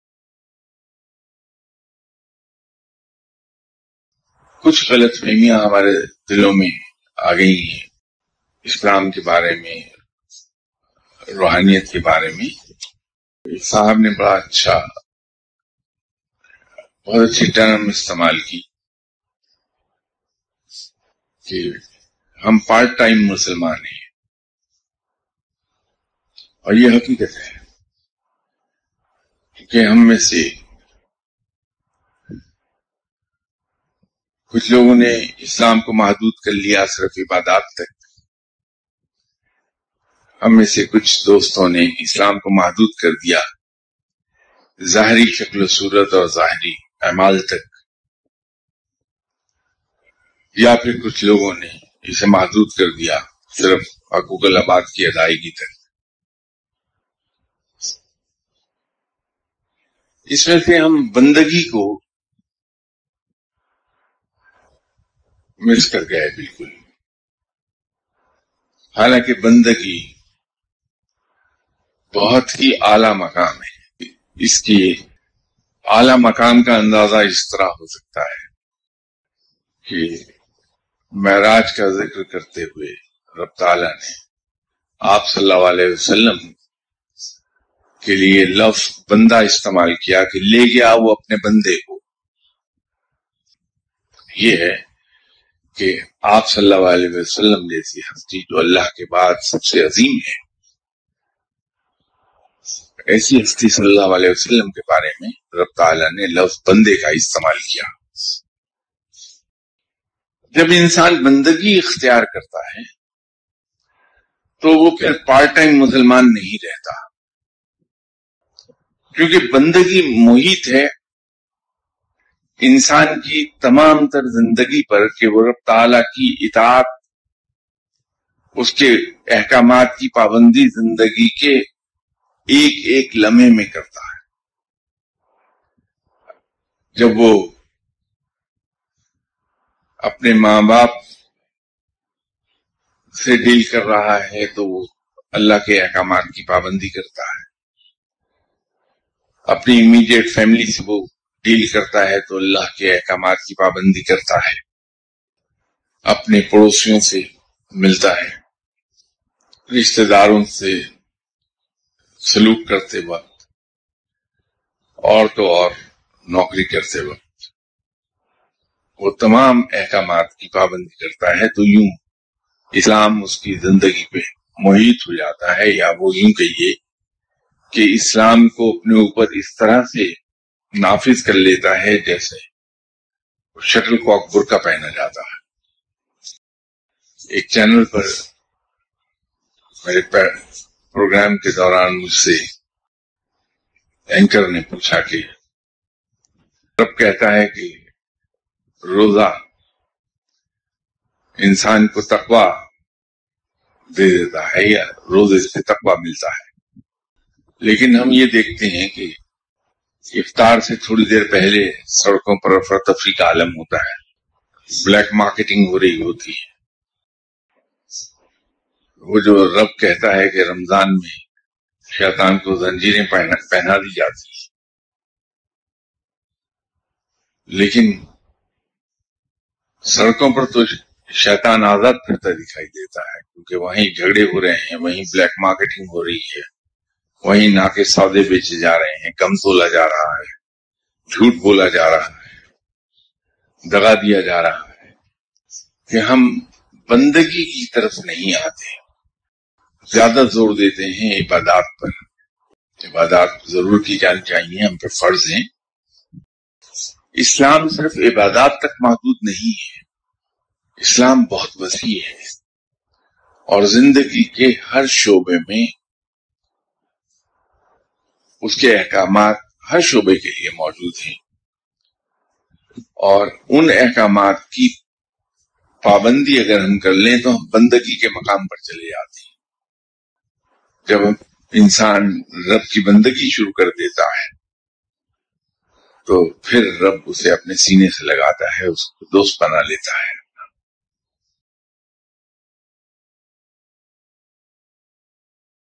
Ibadat Ke Sath Bandagi Bhi Zaroori Hai - by Qibla Syed Sarfraz Ahmad Shah Sahab
Excerpt from Arzang e Faqeer Book - Chapter 2- Page 34

Visit Syed Sarfraz Ahmad Shah official website to read and listen all books for free